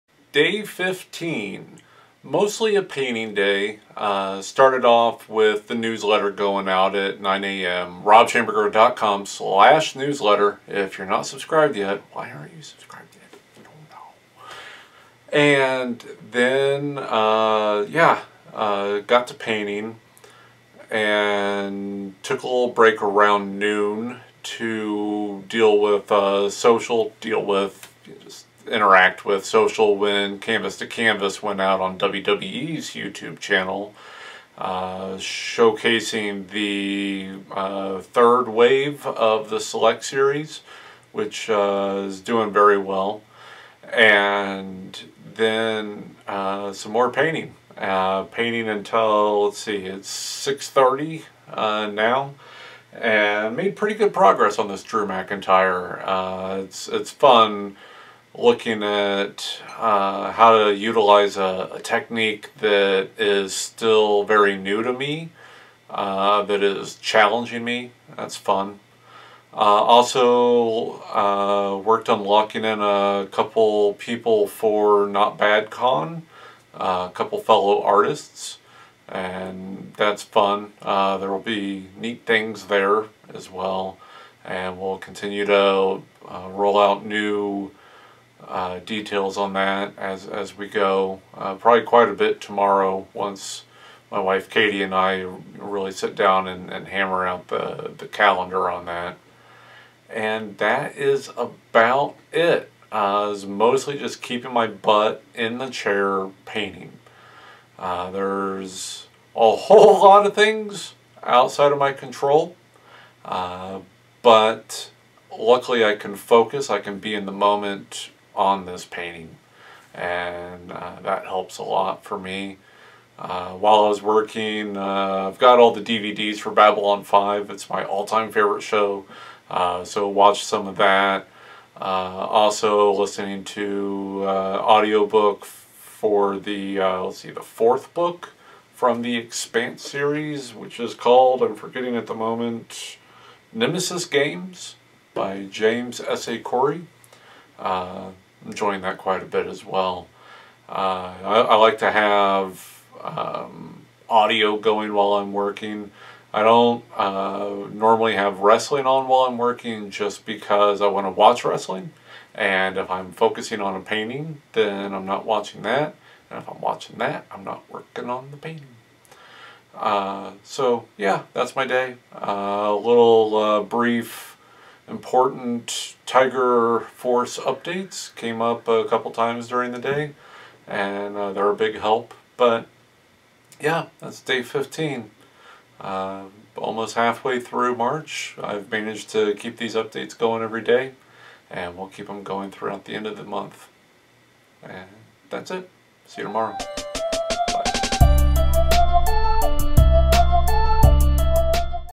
Rob Schamberger's Daily WrestleMania Vlog, Day Fifteen!
After getting my weekly email newsletter out and engaging on social media once @WWE Canvas 2 Canvas published, I got back to work on my new Drew McIntyre portrait. Almost done! Plus more things getting locked in on this week's Not Bad Con!
Leading into WrestleMania Axxess I'm doing a daily update on how much work I have to do and how little time I have left to do it. Currently at 7 paintings with 11 working days. I choo-choo-choose you!